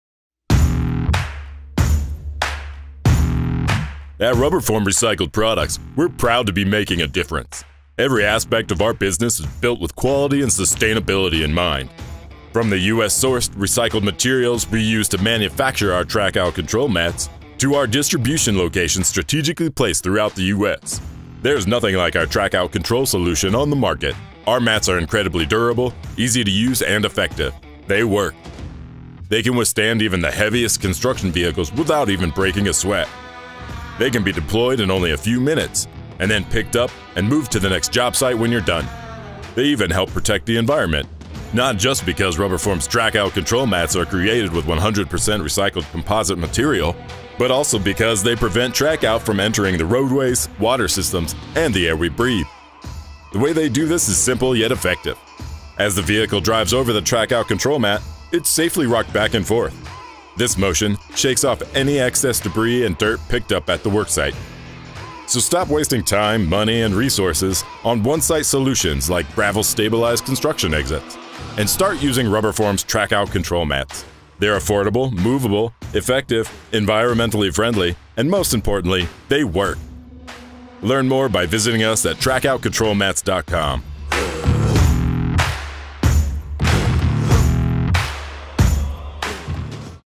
RubberForm's Trackout Control Mat Overview
RubberForm's Track-out Control Mat Overview, the best Track-out mat on the Planet because it WORKS!